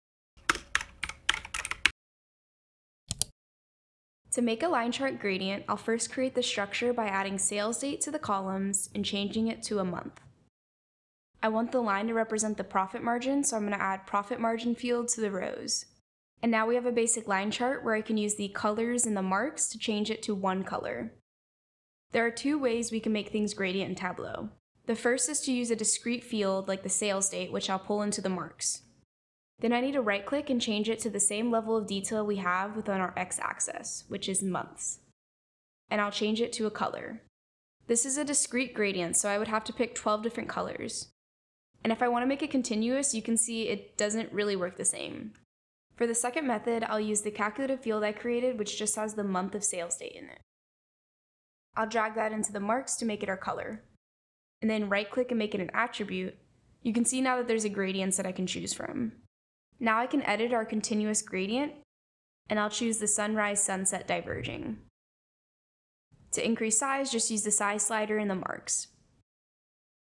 2 Methods for Gradient Line Charts in Tableau in Just 1 Minute!
Learn how to create gradient line charts in Tableau using both discrete and continuous methods in just one minute! This quick guide will show you two powerful techniques to enhance your data visualizations with stunning gradient effects. Perfect for anyone looking to quickly upgrade their Tableau skills.